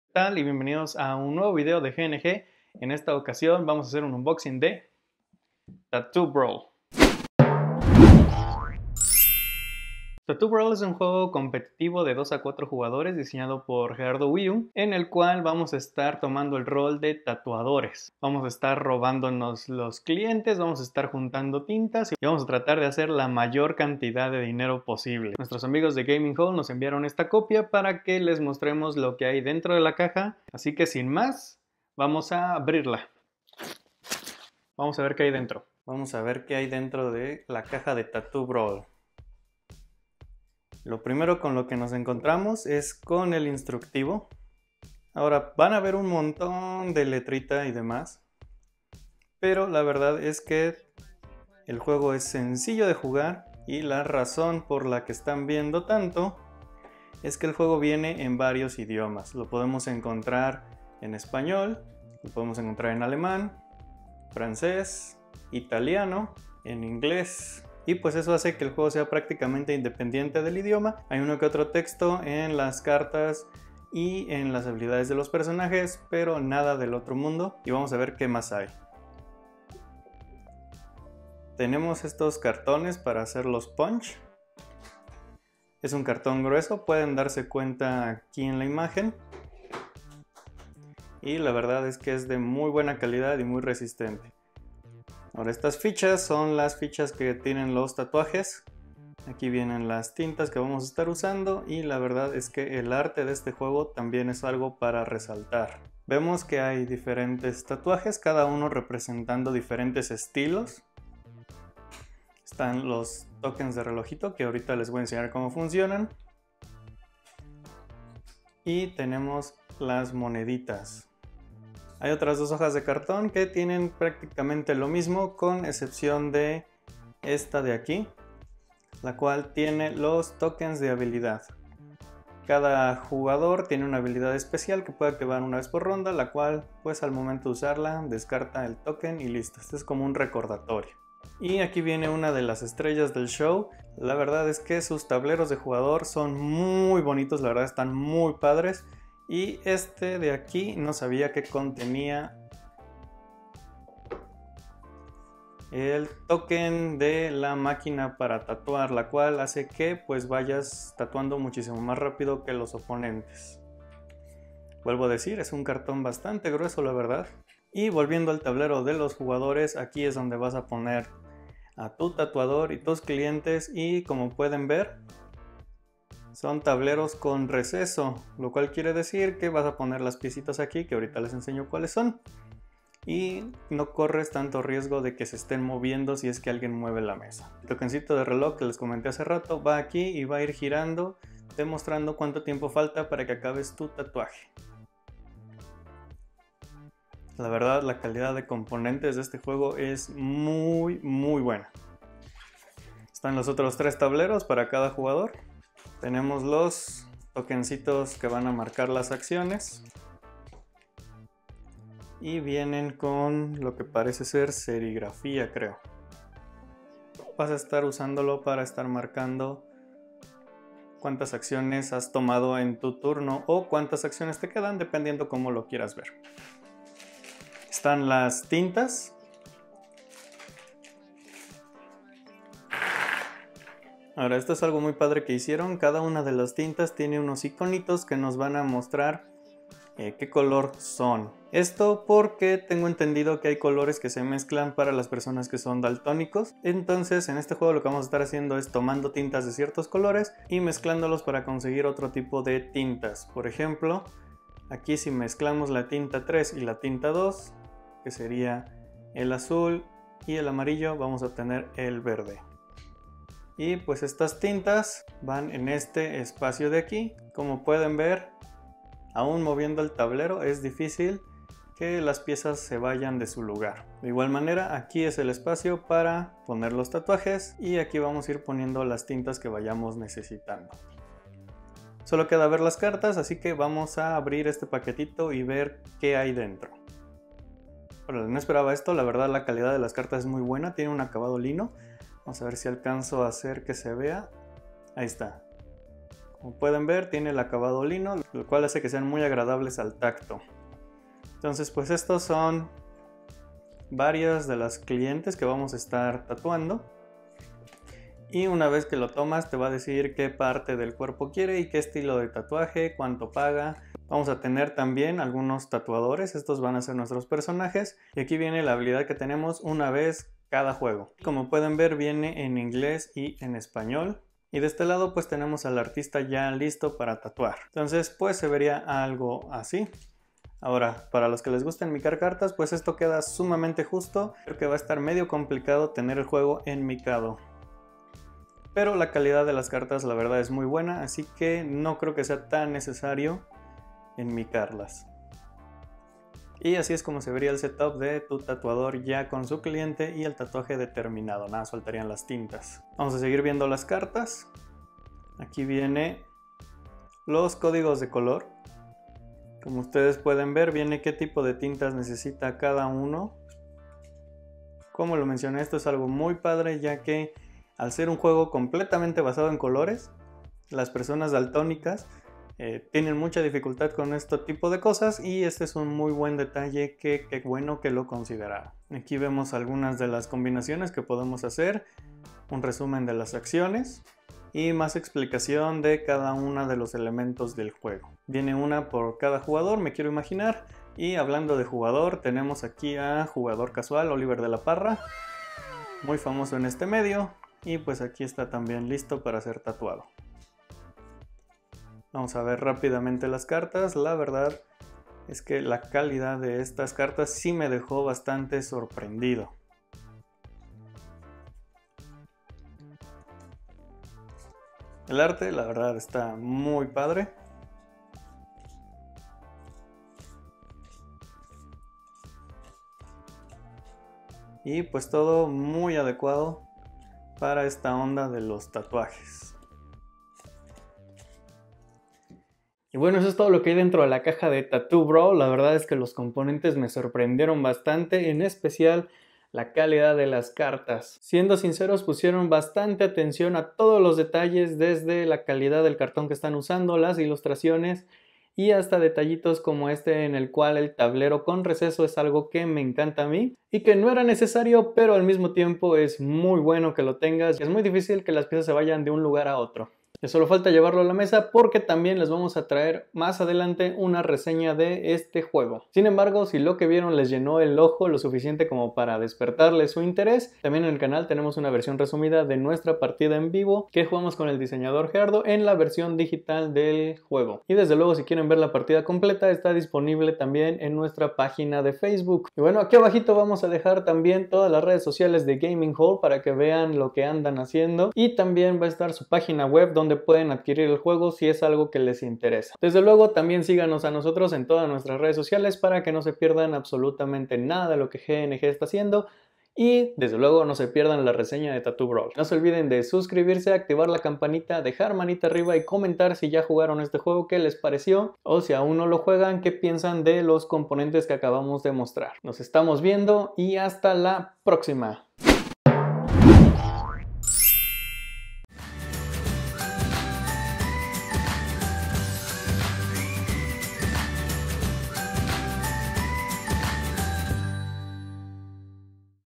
Tattoo Brawl [Unboxing]
¡Josh nos muestra qué hay dentro de la caja de Tattoo Brawl!
Tattoo Brawl es un juego mexicano donde estarás buscando conseguir más puntos de victoria que tus oponentes, todo esto por medio de diferentes tatuajes y clientes.